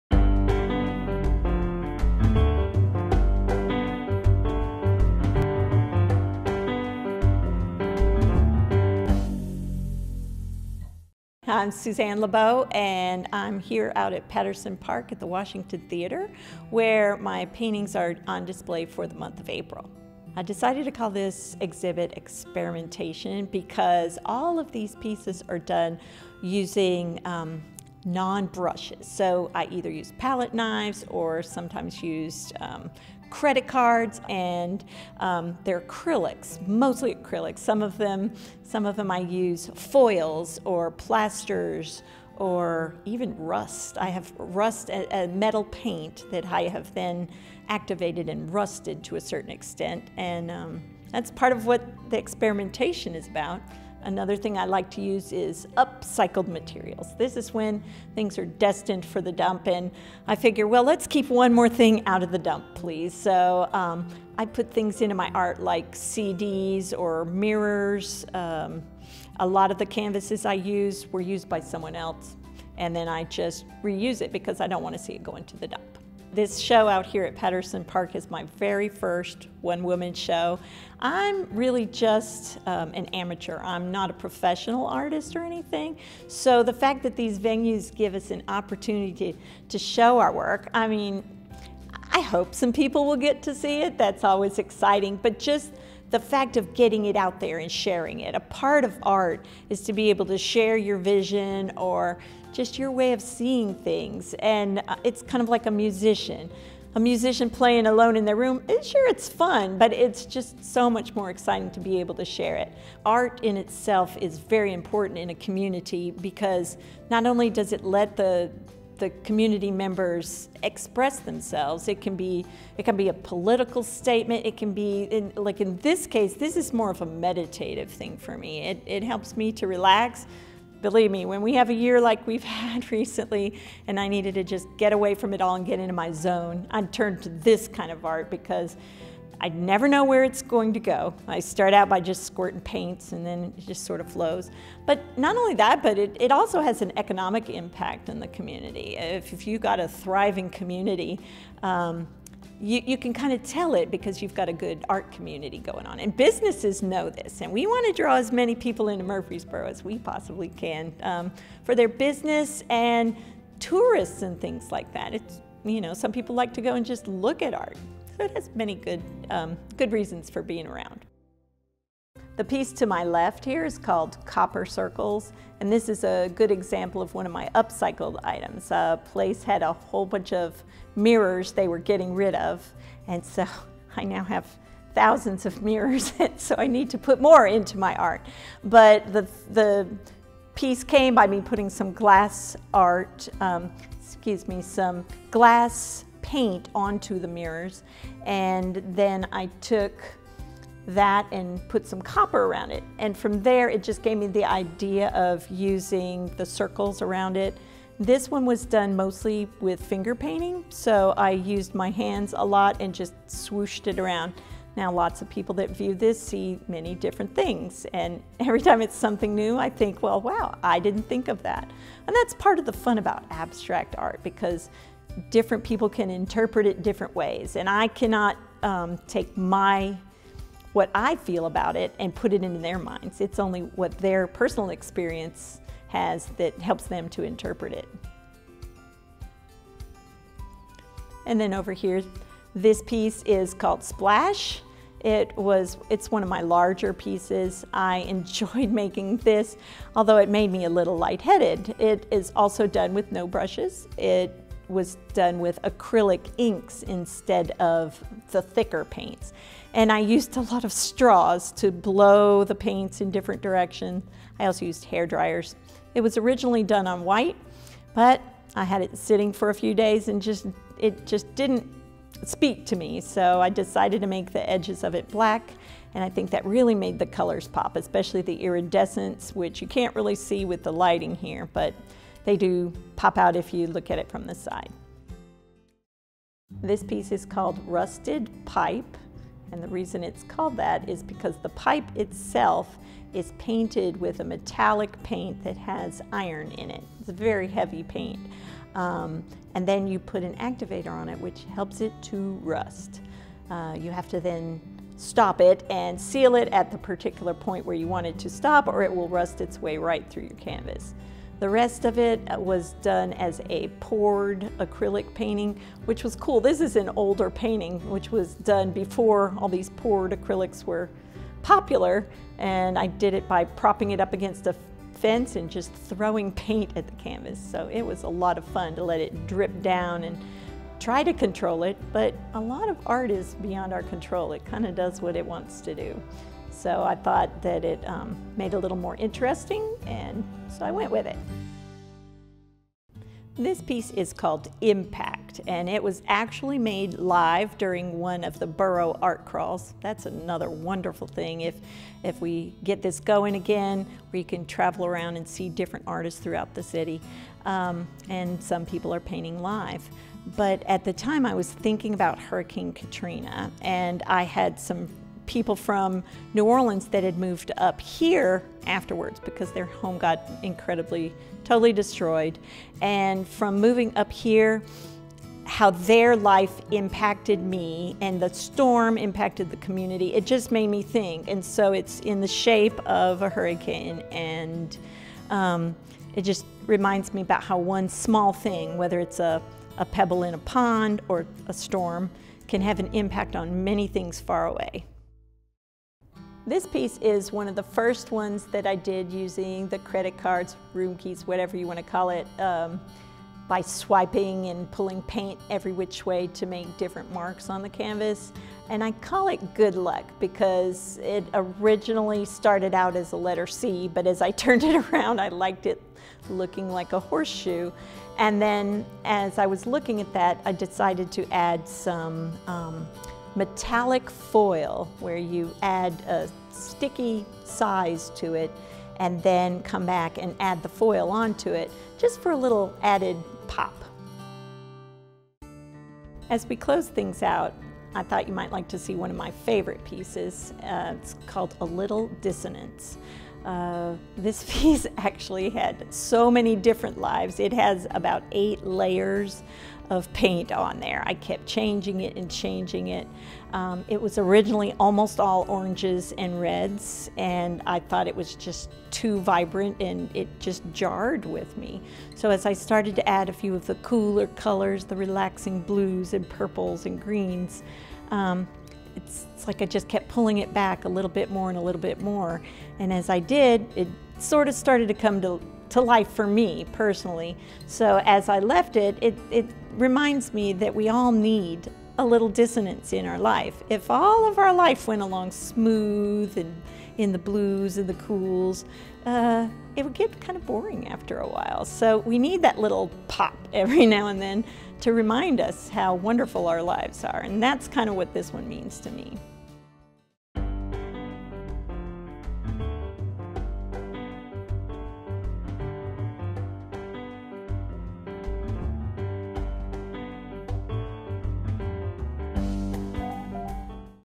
Arts Break-Suzanne LeBeau Artwork
The artwork of Suzanne LeBeau is featured at the Community Gallery at the Washington Theatre inside Patterson Park.